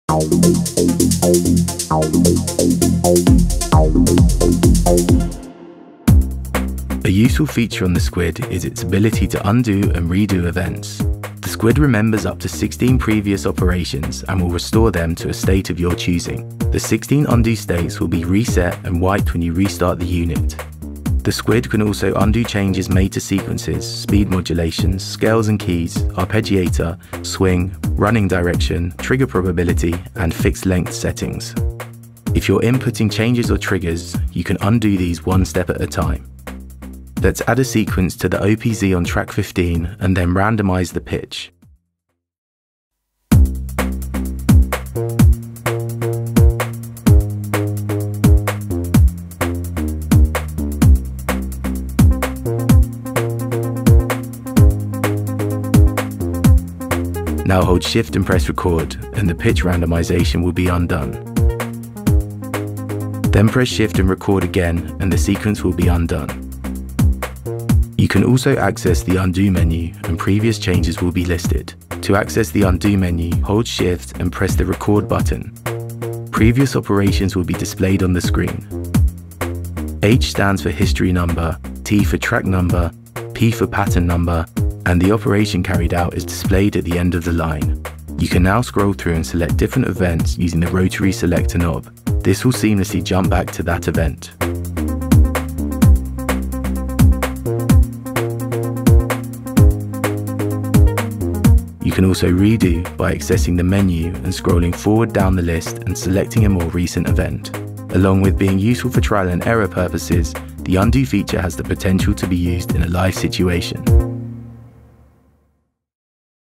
TORAIZ “SQUID” Multitrack Sequencer Tutorials: Undo
Made a mistake and ruined your track? Don’t worry. The SQUID remembers up to 16 previous operations and will restore them to the state you want.